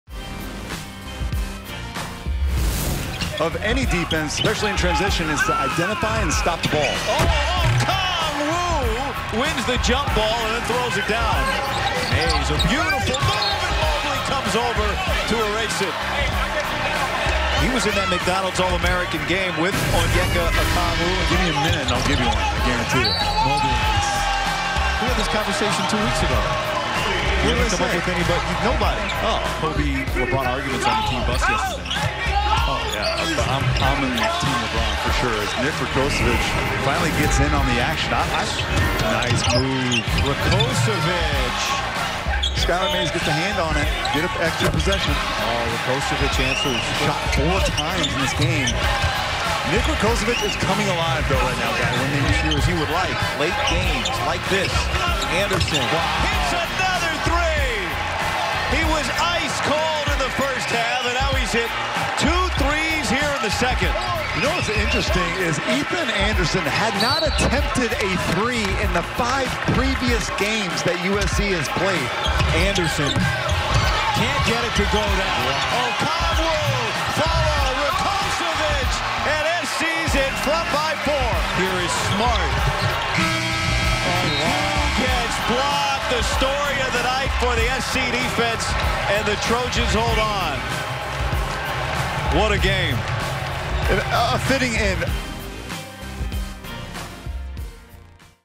Highlights: USC men's basketball finds shooting touch in second half to edge LSU
USC moves to 7-1 all-time against LSU with a 70-68 win over the Tigers on Saturday evening at Staples Center. Five Trojans reached double figures in scoring, led by Jonah Matthews with 15 points. USC shot 50% in the second half, erasing a seven-point halftime deficit for a big win over an SEC foe.